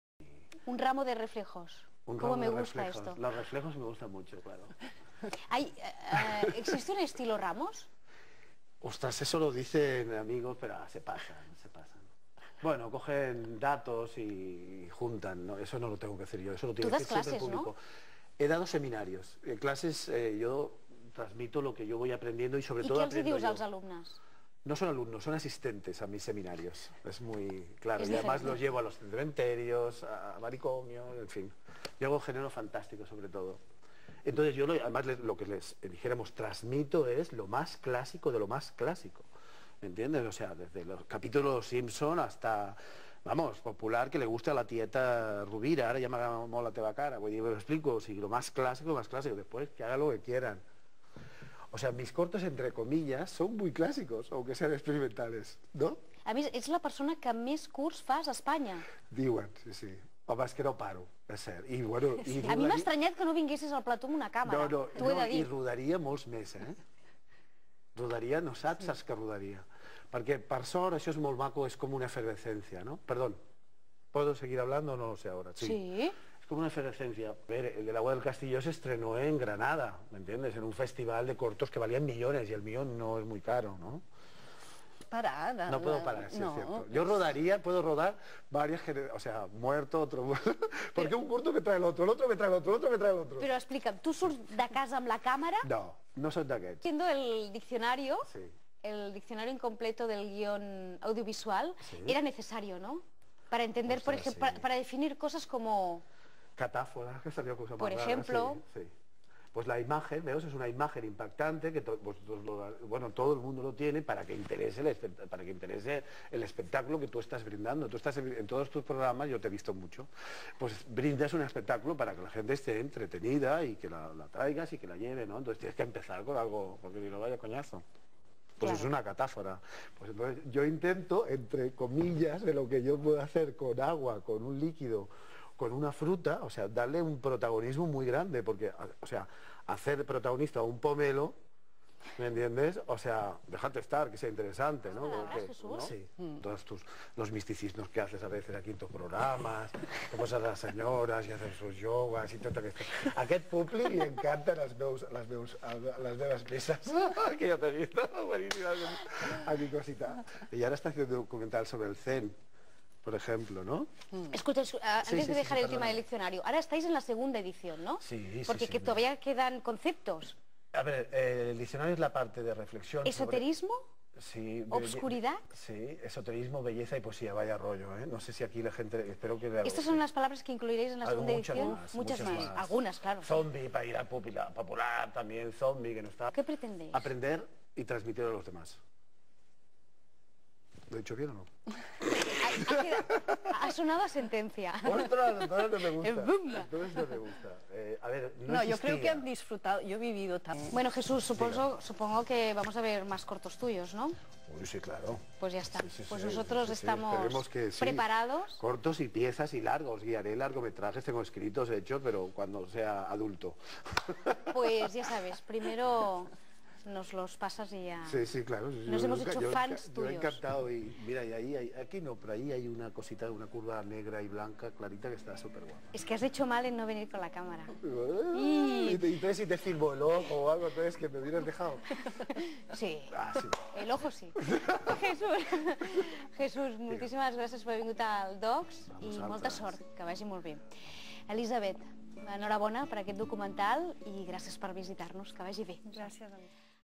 Jesús Ramos
Jesús Ramos entrevistado por Olga Valencia ( extracto de la entrevista  que se le realiza junto a Elishabet Prandi). Ramos habla de sus cortos, de sus videocreaciones y de su Diccionario del guion.